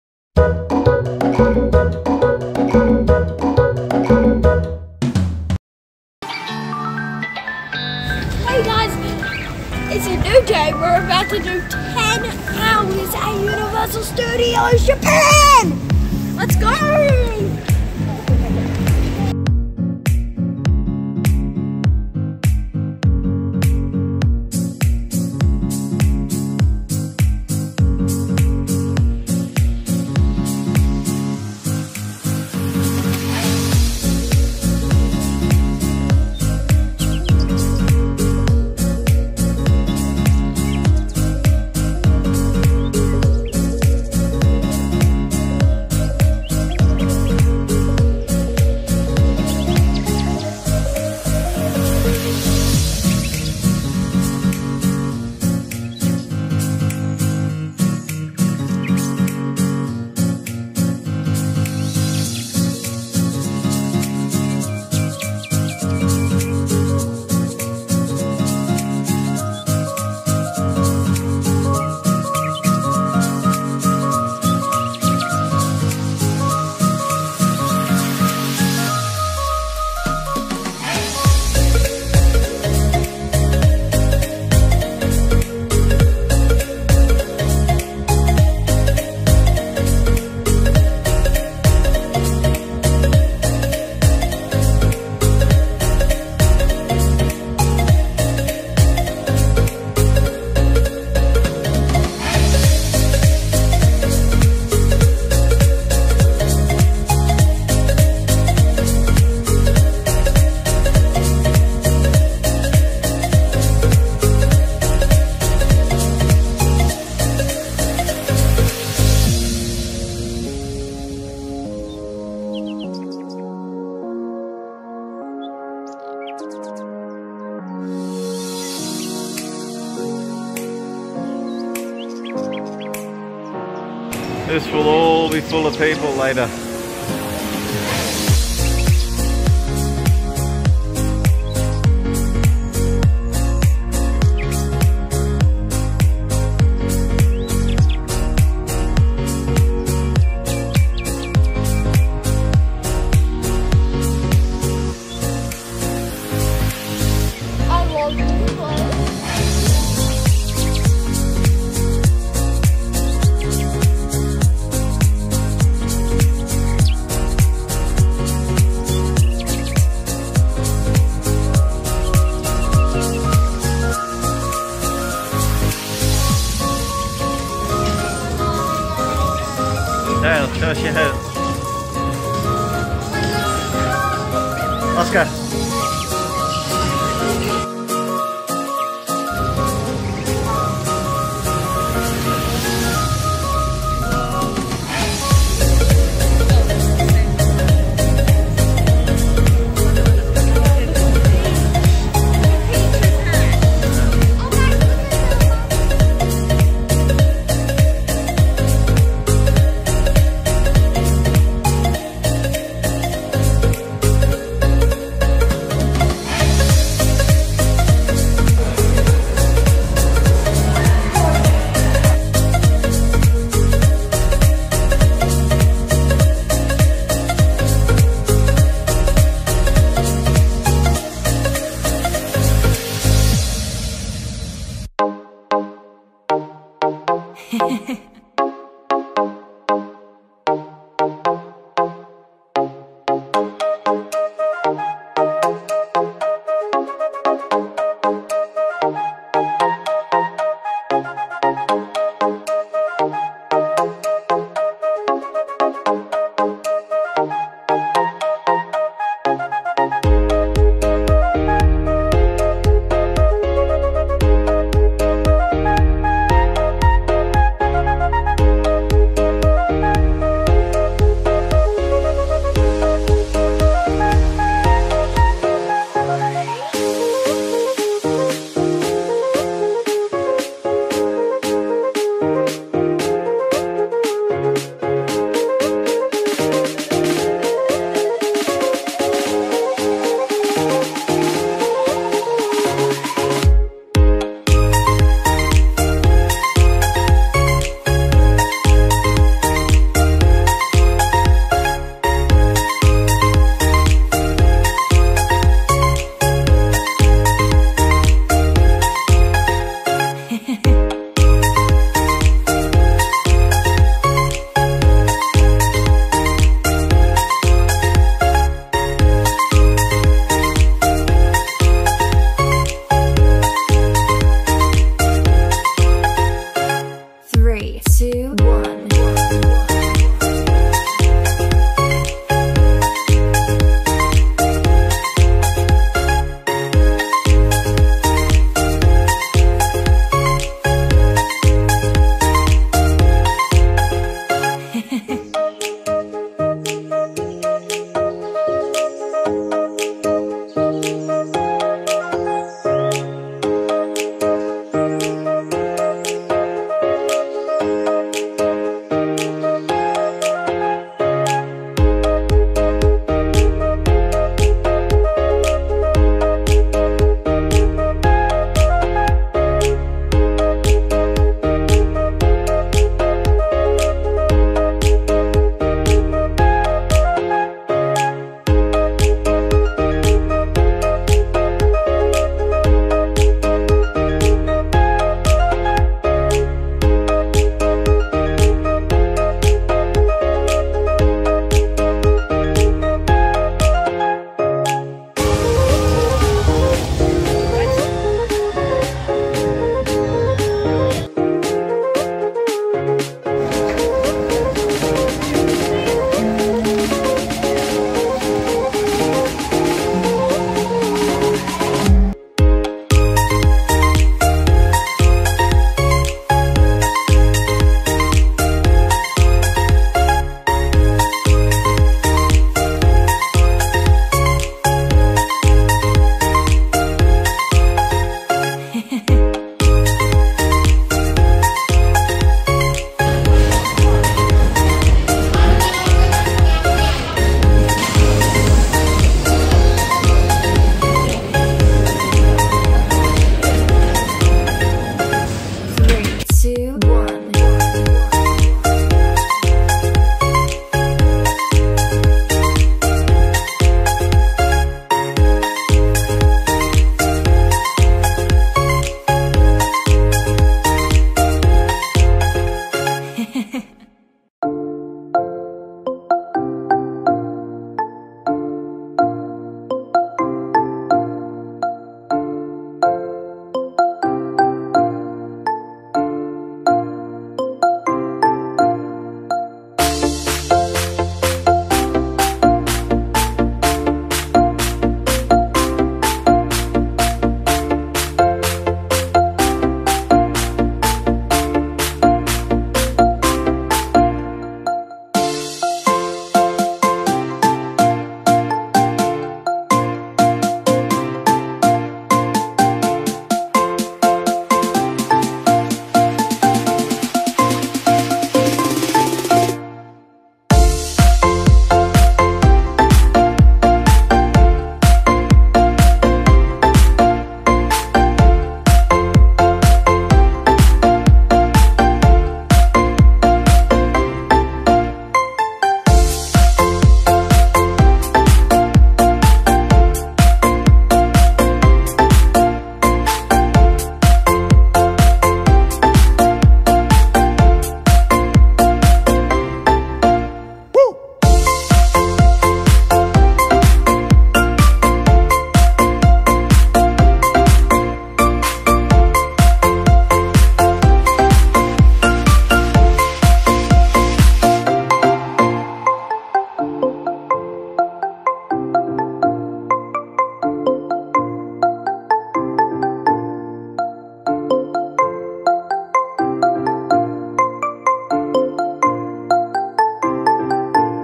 Dale Discovers - Japan 23 - Universal Studios Japan
Dale visits USJ for a full day to explore.